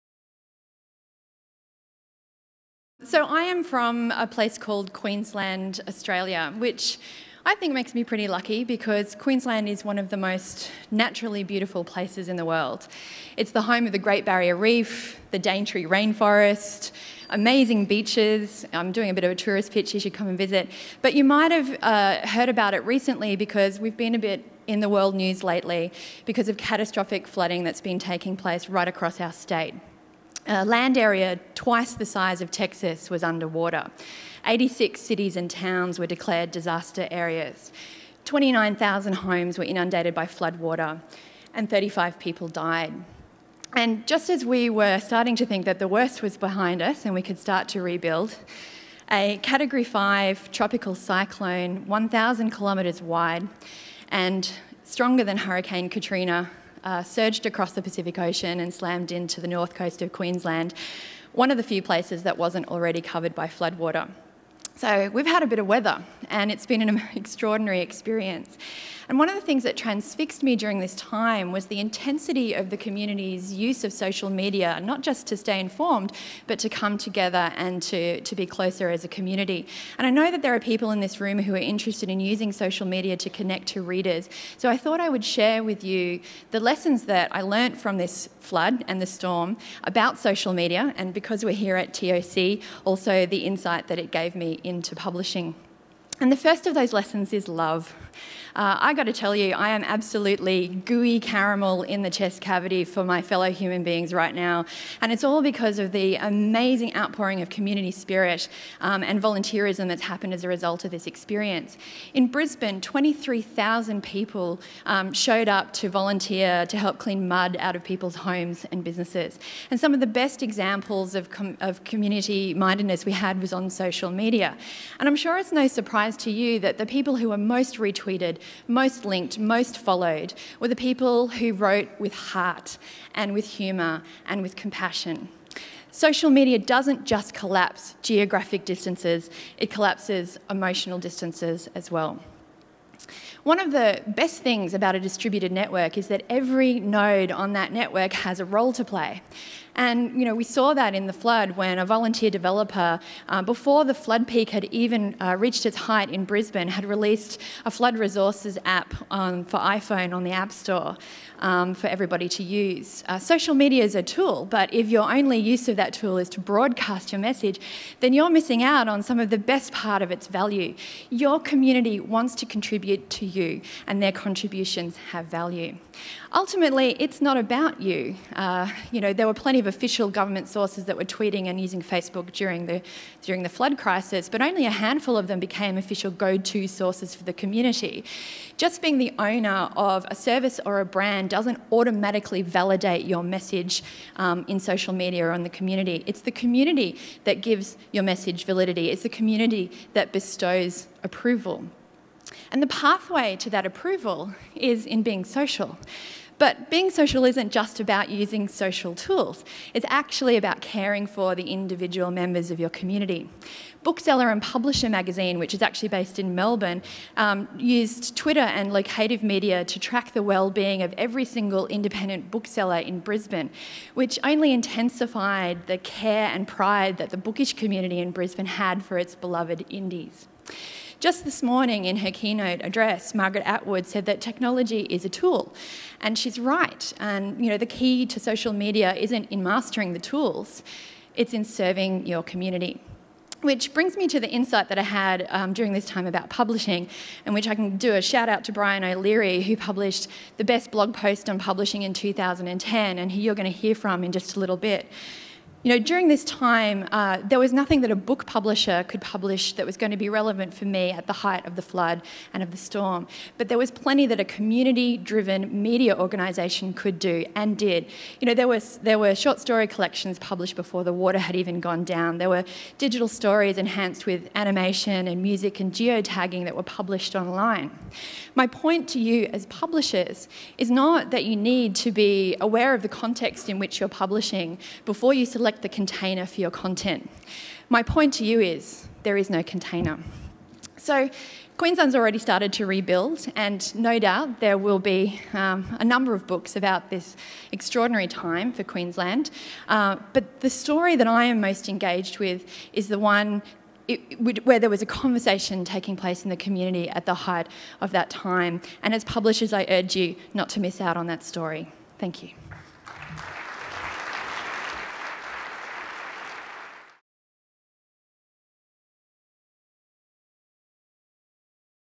TOC 2011: Kate Eltham, "Four Things a Catastrophic Flood Taught Me..."
"Four Things a Catastrophic Flood Taught Me About Social Media...(and One Thing It Taught me About Publishing)"

Kate Eltham

Queensland Writers Centre

Kate Eltham is the Chief Executive Officer of Queensland Writers Centre and founder of if:book Australia, a think-and-do tank exploring book futures. In 2009 she was named by The Australian newspaper as one of 10 Emerging Leaders of Culture.